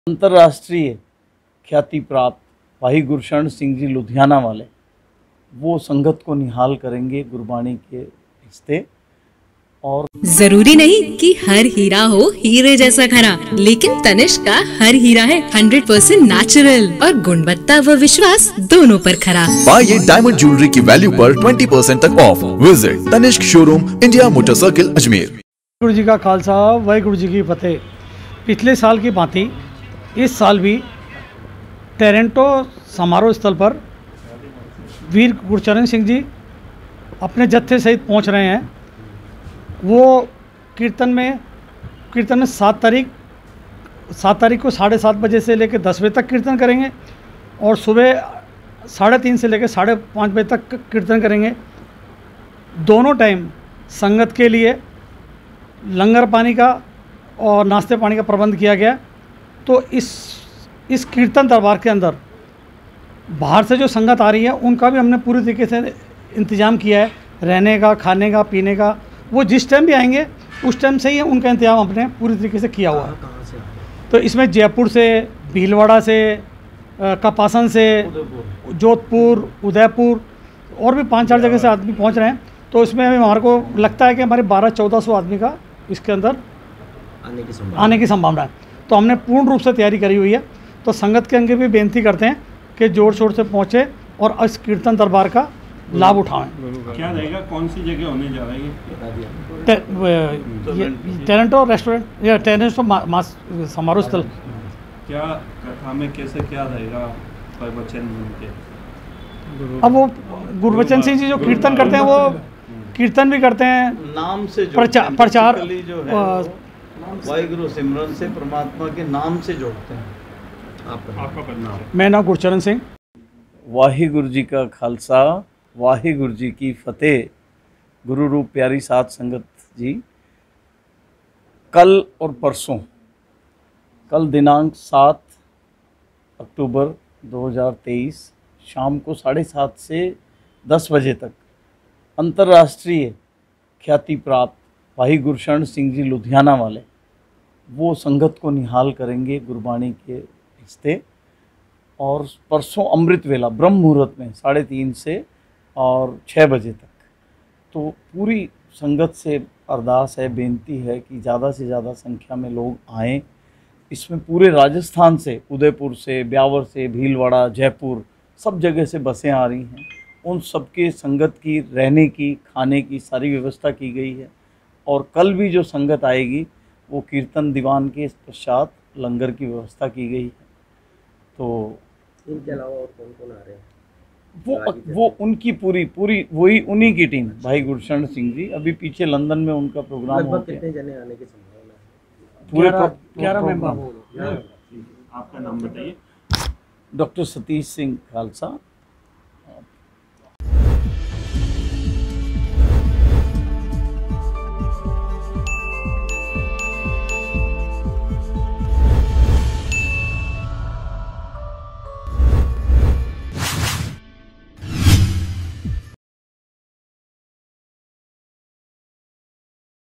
Ajmer विशेष अमृतवेला समागम का आयोजन | MTTV INDIA
विशेष अमृतवेला समागम का आयोजन
कार्यक्रम टोरंटो गार्डन 9 नंबर पेट्रोल पंप नसीराबाद में होगा आयोजित
कार्यक्रम में लुधियाना वाले गुरशरन सिंह पहुंचेंगे